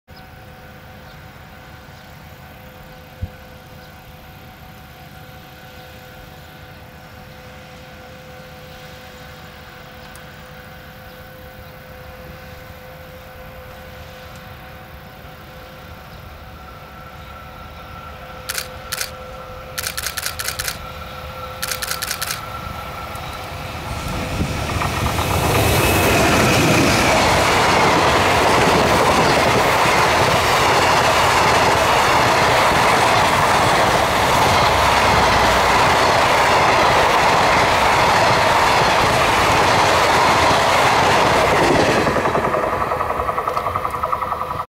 宇都宮線 貨物 EH500-25 3054レ 間々田～野木 2013.09.22
黒磯訓練のちょい前に通過しました。札幌からの貨物です。もうちょいで隅田川タです。